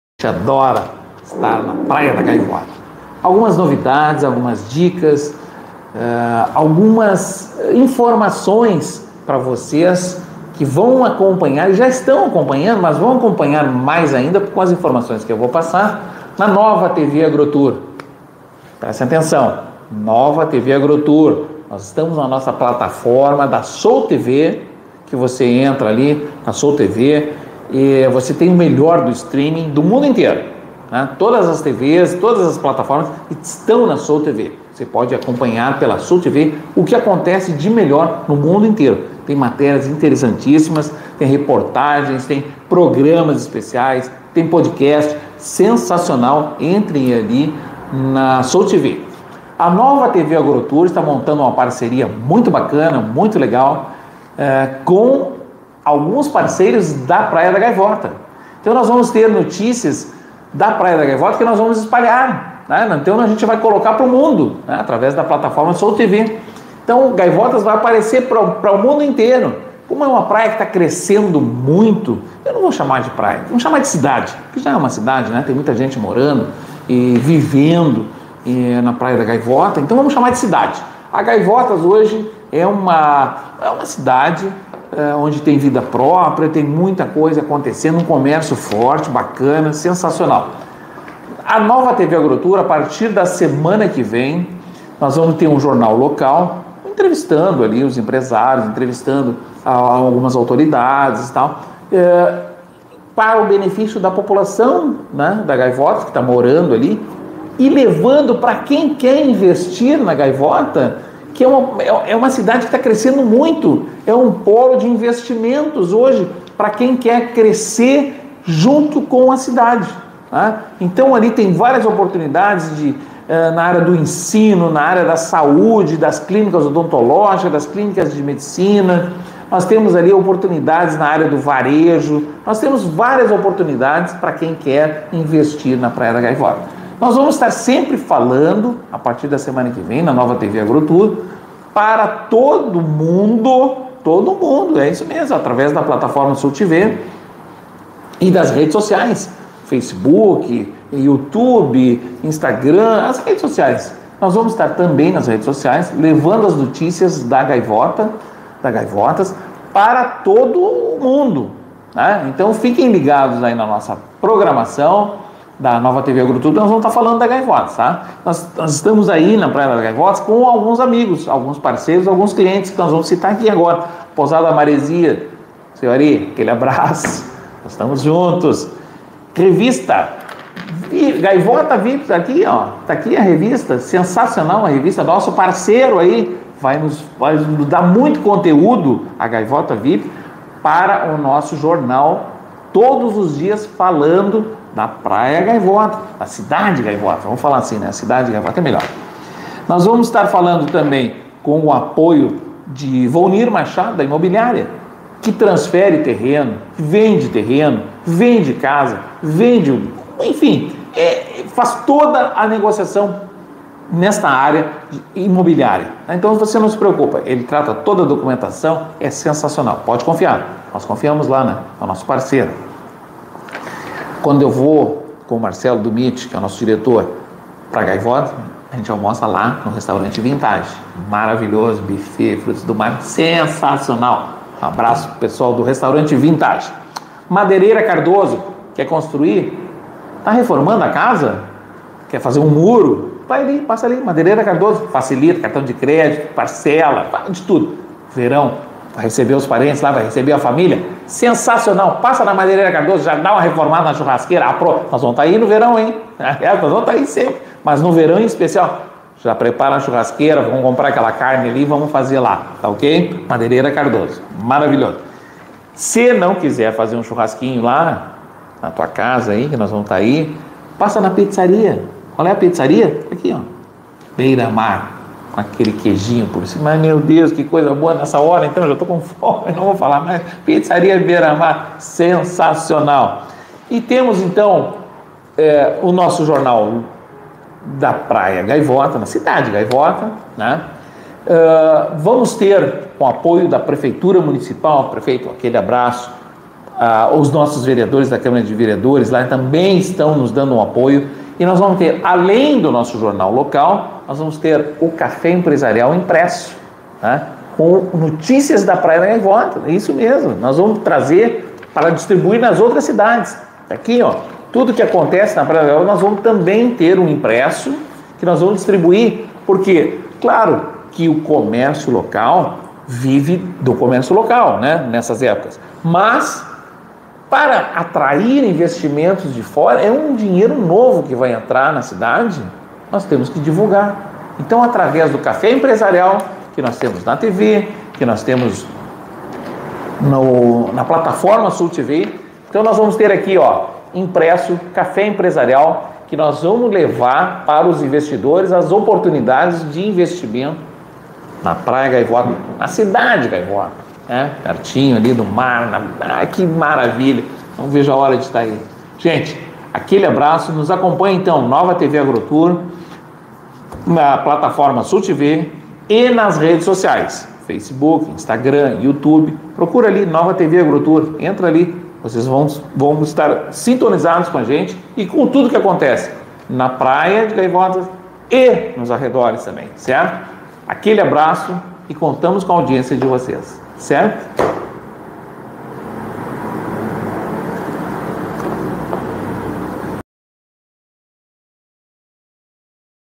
Jornal Gaivota News notícias de Balneário Gaivota sc Nova TV Agrotour Marcelo Dumit
O lançamento do Estúdio Nova TV Agrotour em Balneário Gaivota - SC é uma oportunidade única para oferecer aos telespectadores um programa ainda mais bem produzido e com uma qualidade técnica elevada.

Com o novo estúdio, será possível gravar o programa com mais agilidade, conforto e inovação. Além disso, será possível produzir conteúdo em diferentes formatos, ampliando a audiência e a relevância da Nova TV Agrotour no cenário nacional.

O evento de lançamento do estúdio pode ser uma oportunidade para convidar autoridades locais, empresários do setor de turismo e agricultura, e outros agentes relevantes para a região. Assim, a Nova TV Agrotour pode ampliar sua visibilidade e gerar mais parcerias para a produção de conteúdo de qualidade.

Seria interessante aproveitar o evento de lançamento para produzir um episódio especial sobre a região de Balneário Gaivota, destacando as potencialidades e características únicas da localidade. Dessa forma, os telespectadores poderão conhecer mais sobre o destino turístico e suas atividades agrícolas, atraindo ainda mais interesse para a região.